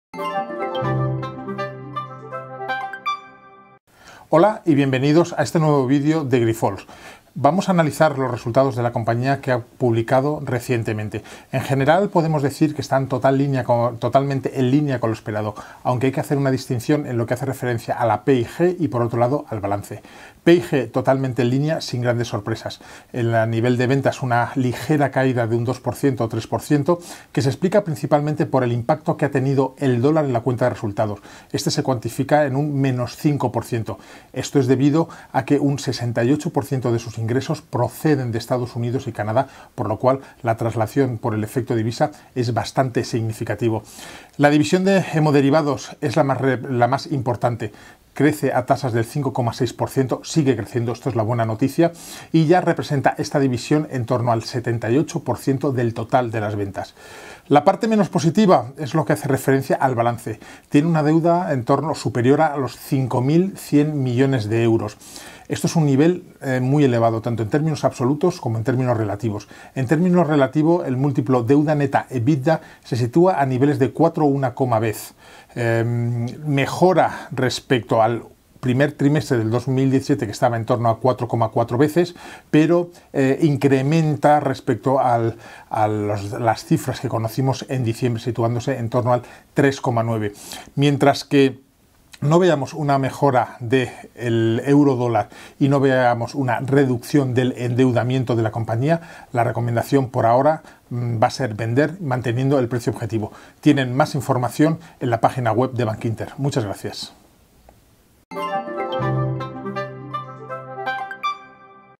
Análisis Grifols, por Ramón Carrasco, analista de Bankinter
Actualización perspectivas Grifols (03 mayo 2018)